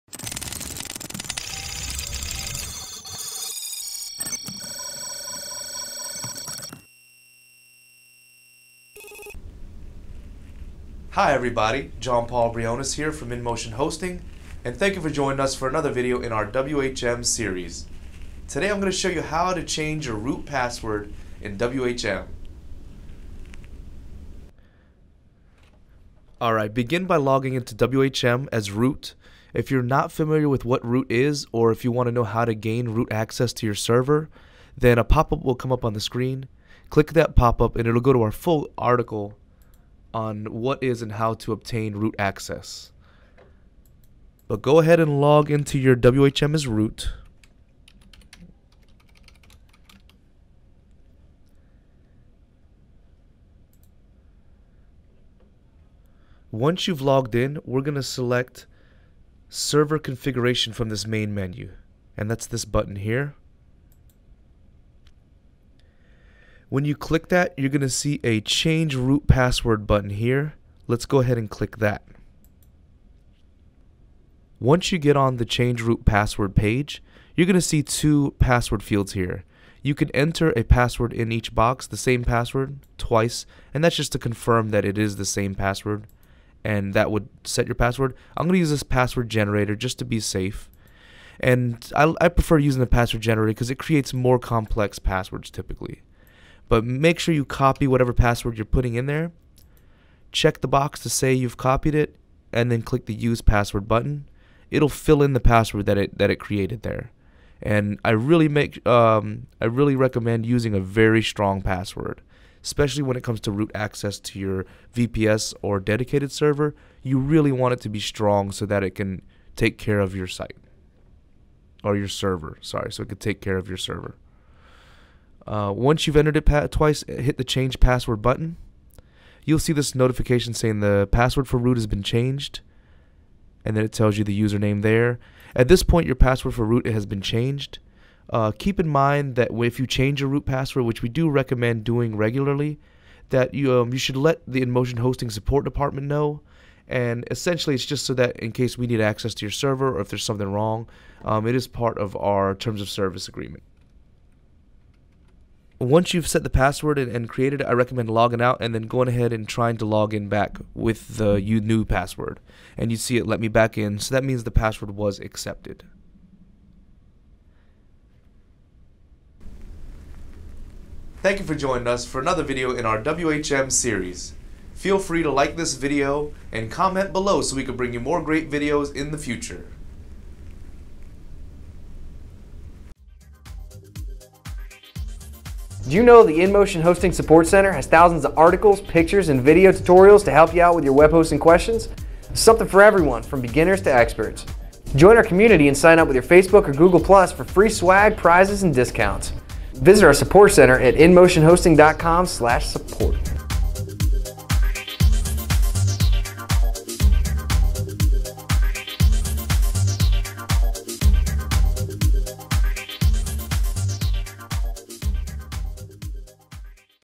How to Change your root Password in WHM
Learn how to Change your root password in WHM, in this walk-through video. Below is an outline of the steps:
1. Login to WHM as root.
2. Click the "Server Configuration" button.
3. Click the "Change Root Password" button.
4. Enter your new root password (twice) and click the "Change Password" button.

Learn more in our full tutorial - How to Change your root Password in WHM

We also have a guide that explains "Root Access and How To Obtain It"